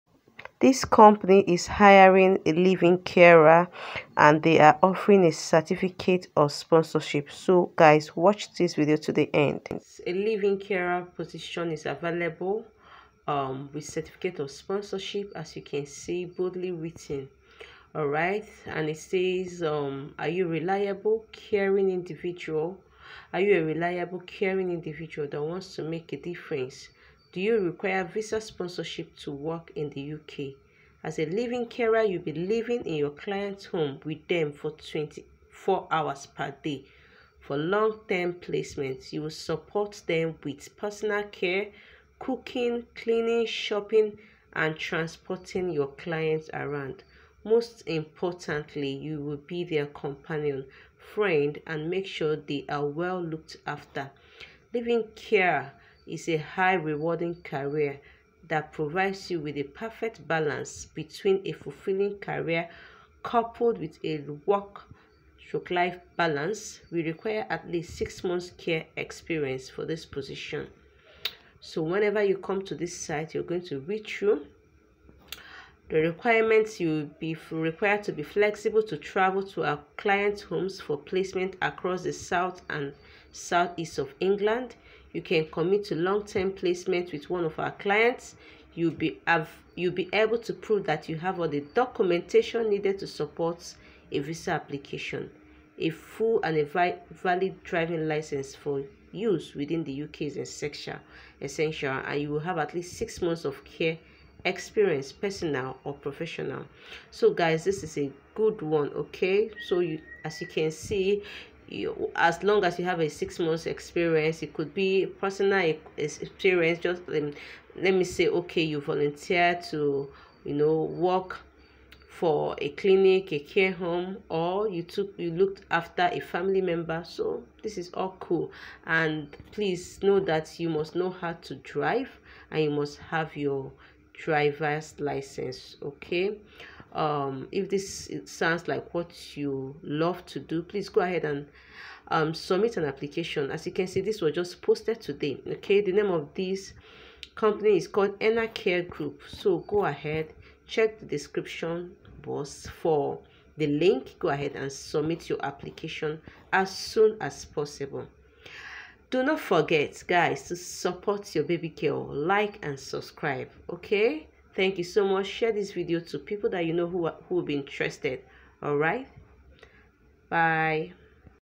This Company is hiring Live-in Carer with Certificate of Sponsorship. Hurry & Apply
Hey lovelies,
This Company is hiring for live in Carers and they are giving certificate and sponsorships to candidates from Overseas.

All my services, from one on one consultation to school application, and writing services